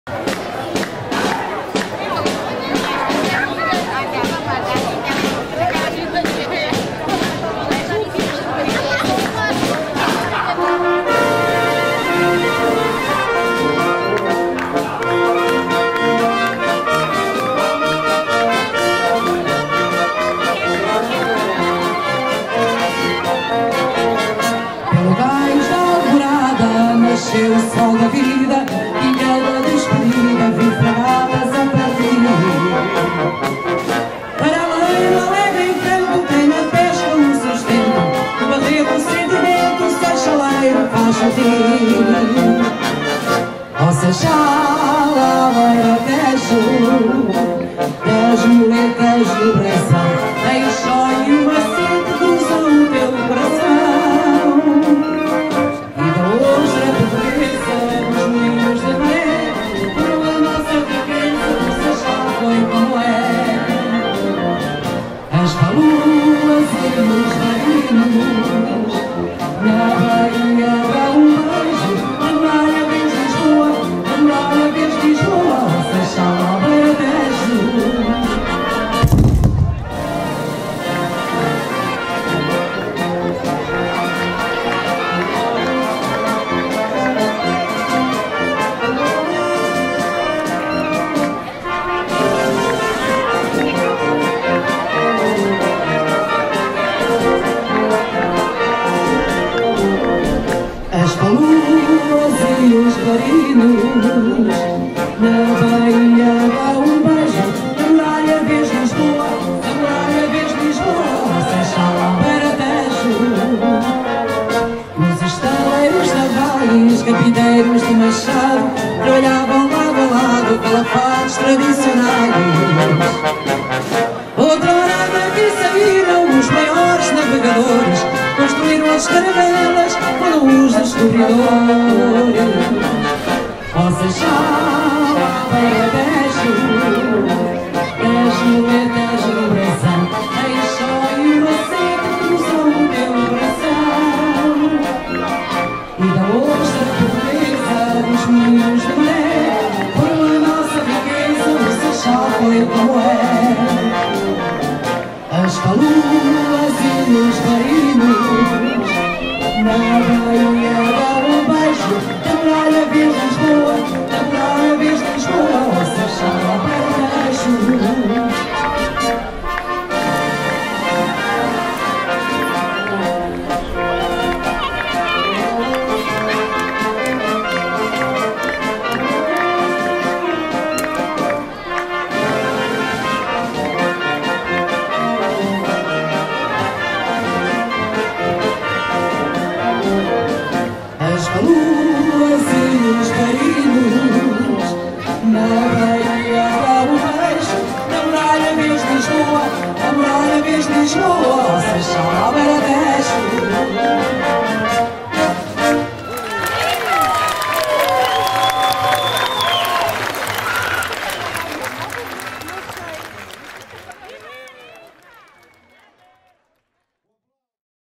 Marcha dos Reformados 2011 - HD720p - Largo da Igreja
Exibição no Largo de Igreja do Seixal, nas festas de S. Pedro 2011. Agradecimento ao Valter Pinho pela gravação e montagem.